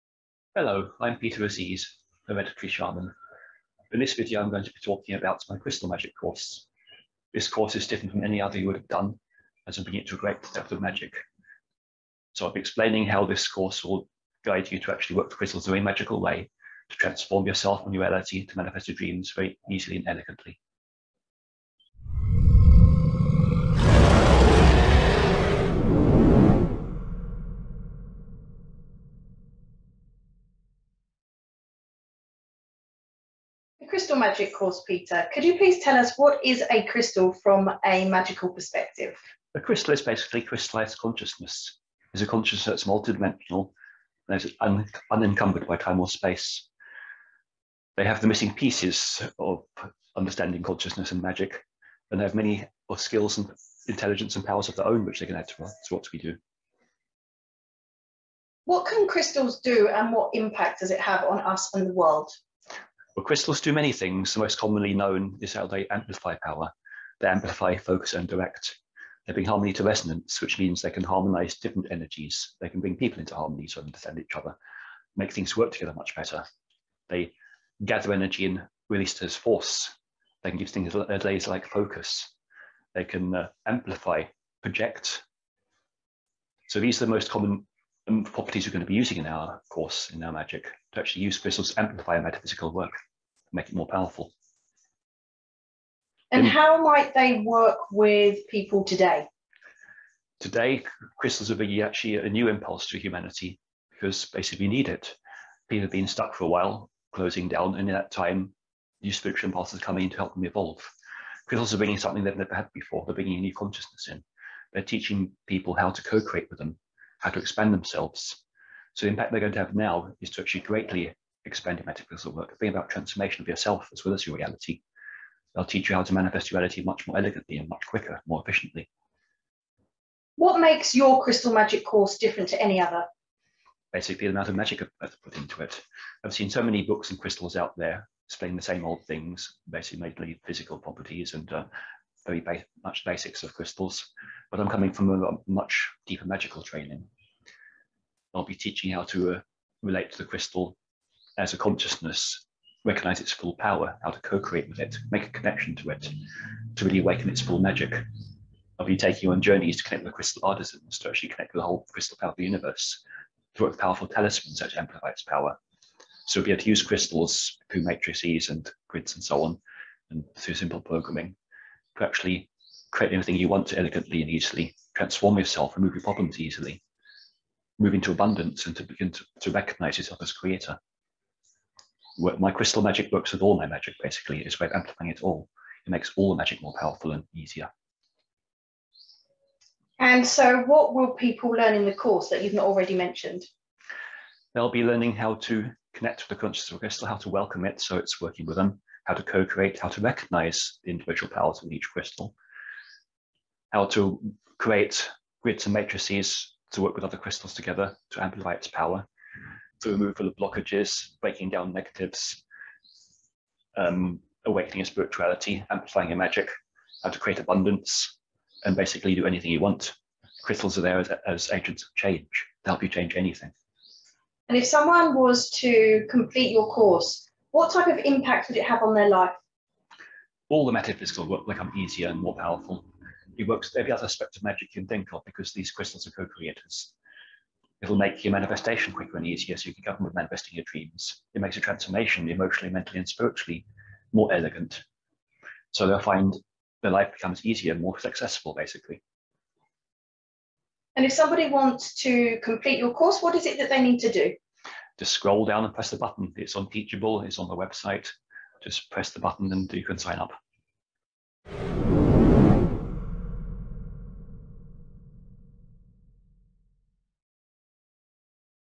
Crystal Magick Course Introduction
In this video I introduce my online course: Crystal Magick
Please see also my recent article Crystal Magick: Amplifying Power, Increasing Your Rate of Success

This course is part of my Magick: The Faerie and Dragon Path series